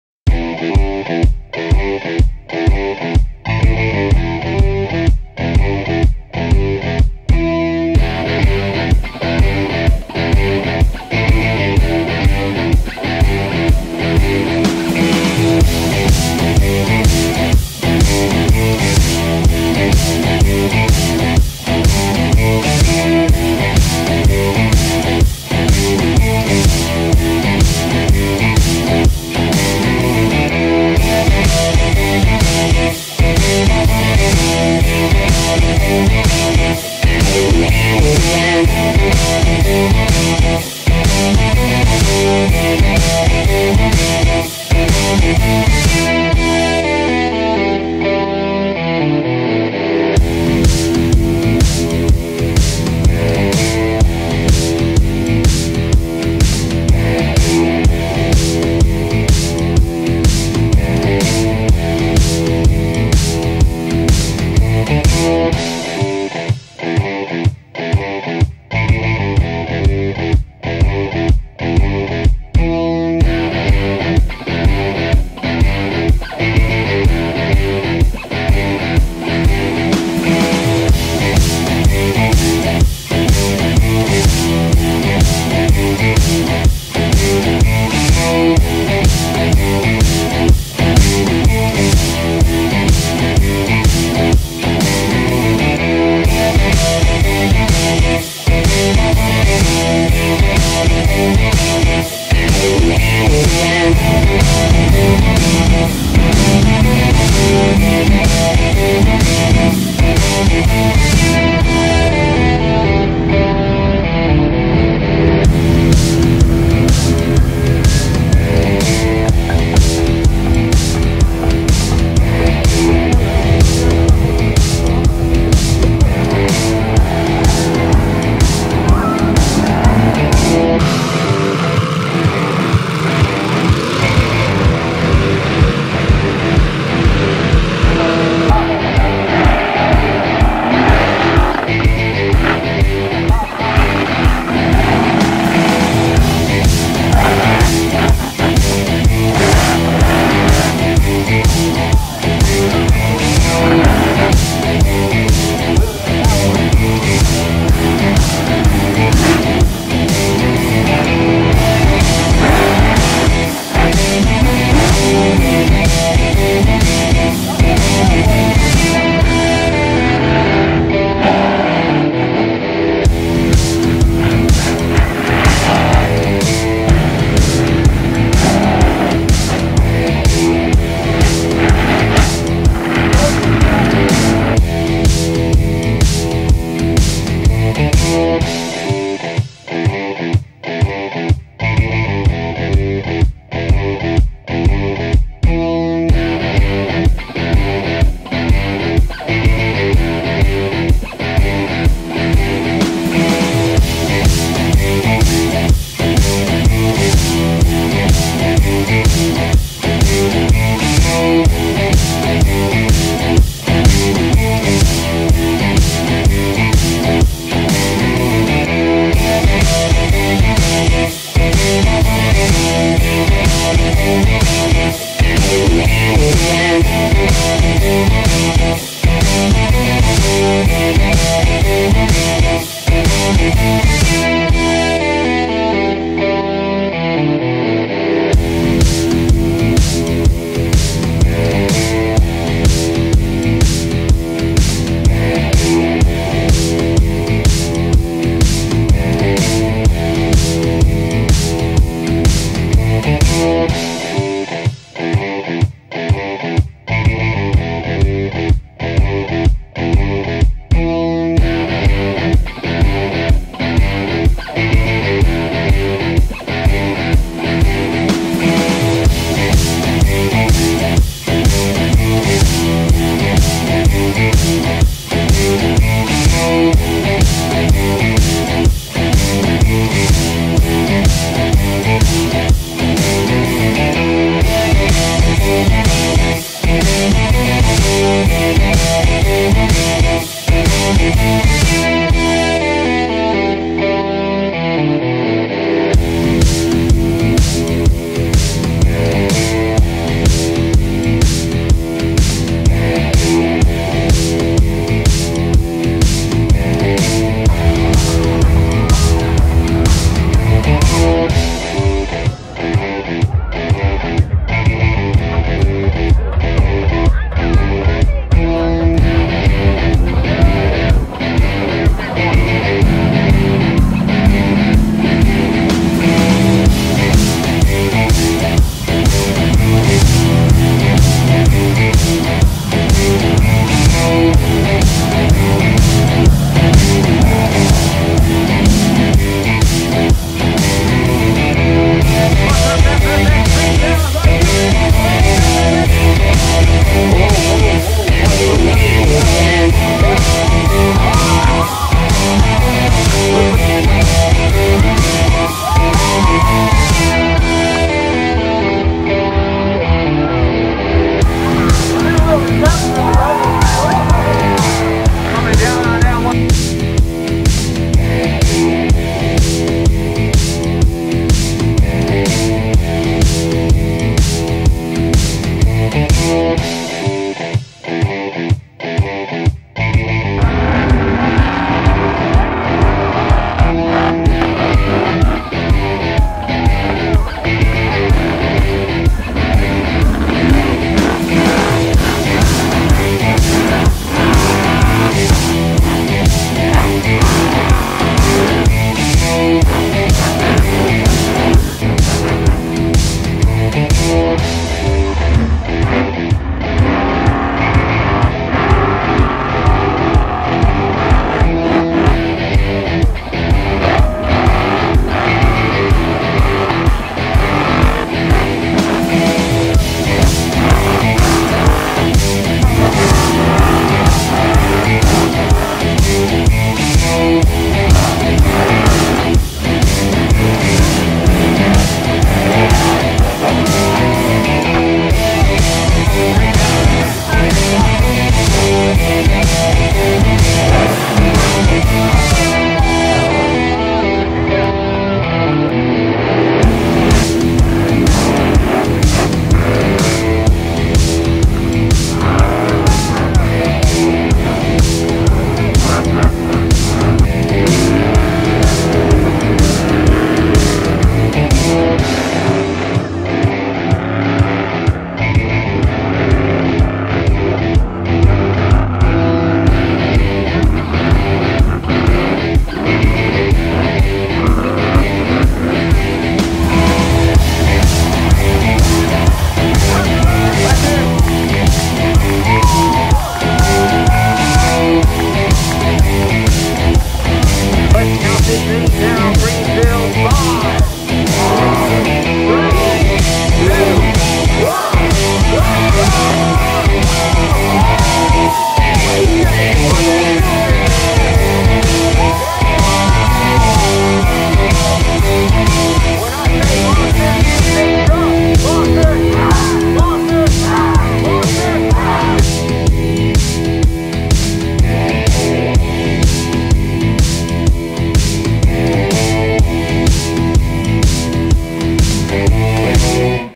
Monster Truck Show 2020, Greenville-Pickens Speedway
Monster Truck at Greenville-Pickens Speedway, Greenville South Carolina